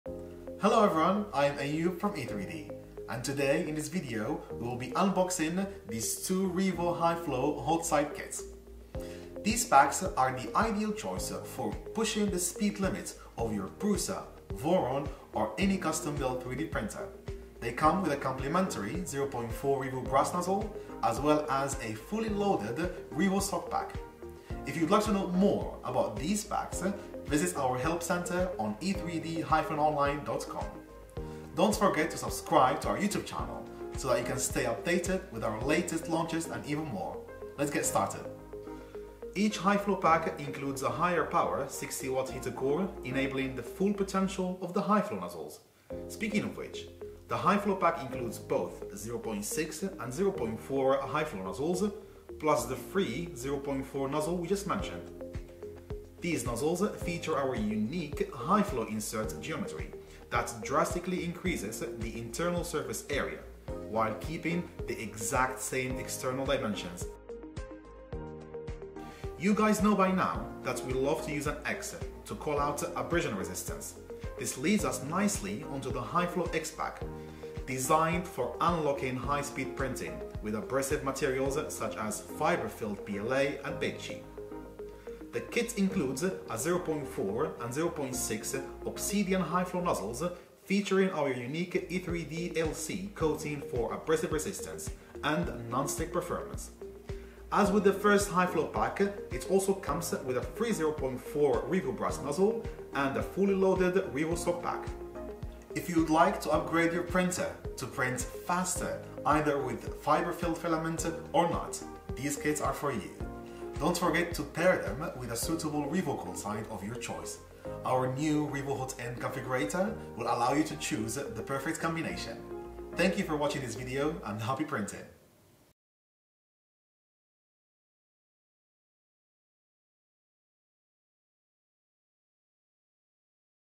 Revo High Flow Pack Unboxing
Push your printer’s print speed capabilities to the limit with our Revo High Flow packs.